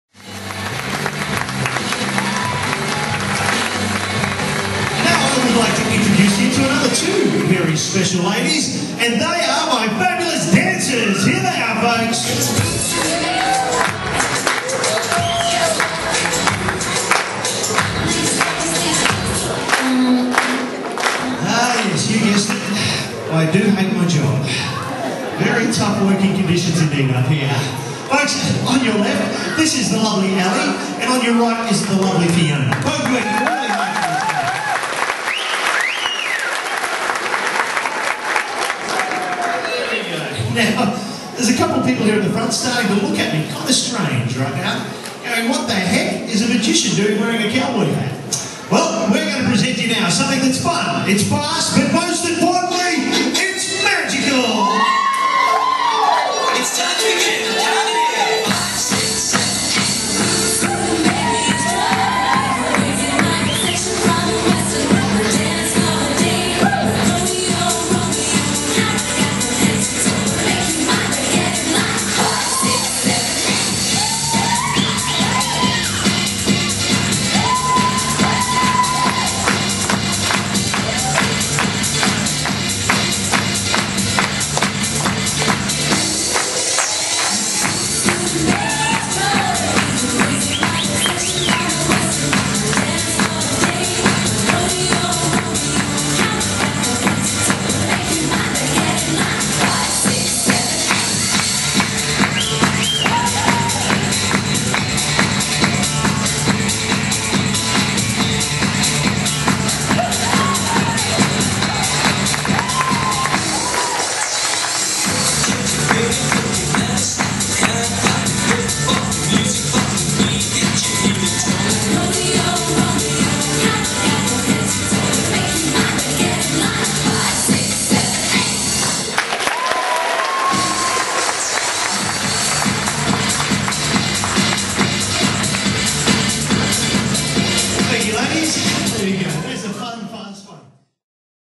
Tony Laffan: Stocks Illusion
The Stocks Illusion is a lighted hearted, fun, theatrical piece that adds light and shade to Tony and Kasey's Illusion show. Not all illusions have to be dark and  serious. It is often a crowd favourite showing a fun side to this magical duo always recieving loud gasps from the audience when the magic of the lighting switch occurs. Friends ahve referred to this presentation as their personality piece.